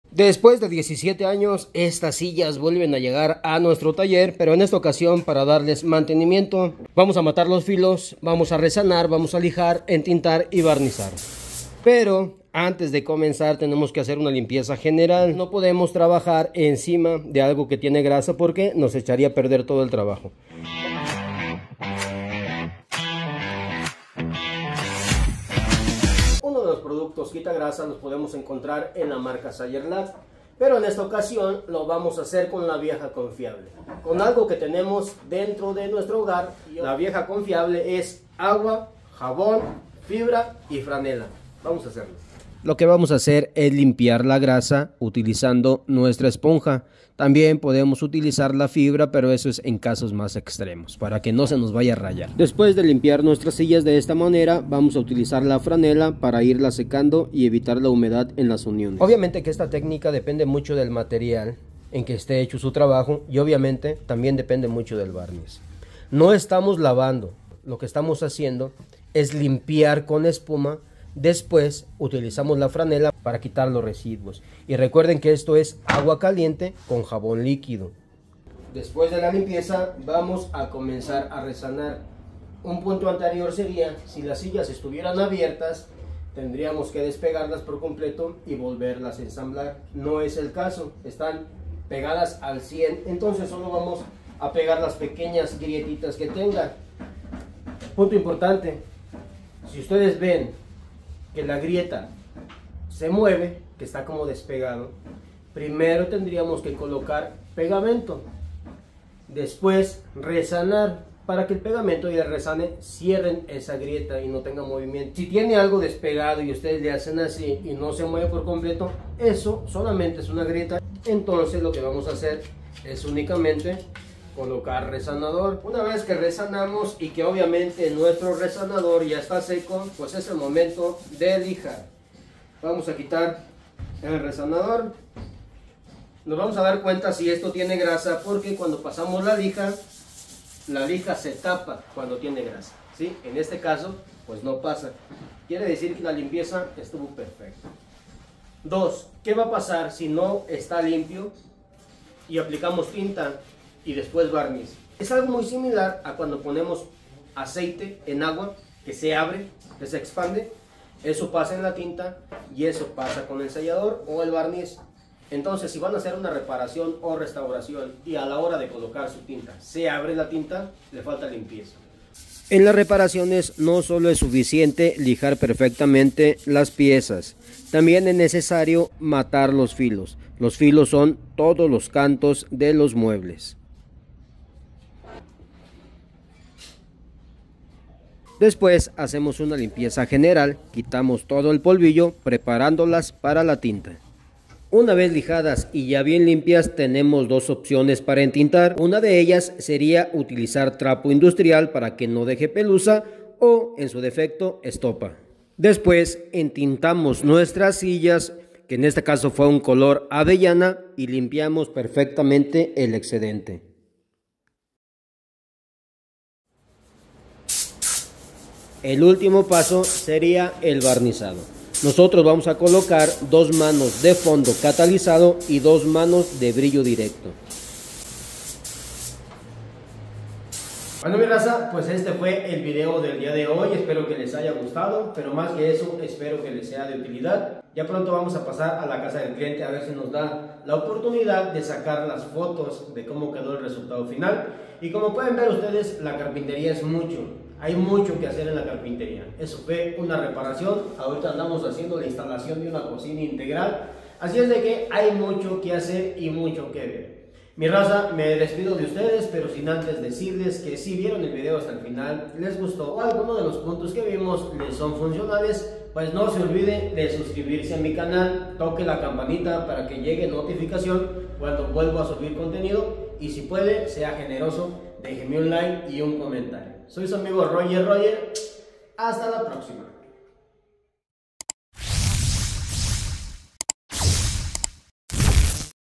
¿Como reparar nuestros muebles? Aprende Carpintería 🫡 TUTORIAL
Las reparaciones son parte de nuestro trabajo en la Carpintería aquí les dejo algunos puntos importantes a considerar al realizarse este tipo de trabajo